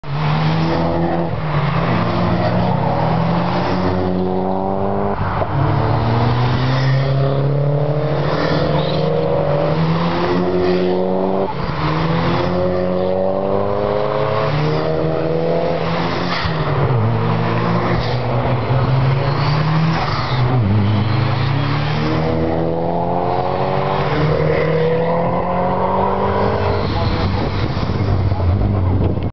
Toyota Celica GT Four Owners Club
Members of the club leave the pits to get on track at the 2007 Rallyday in Castle Combe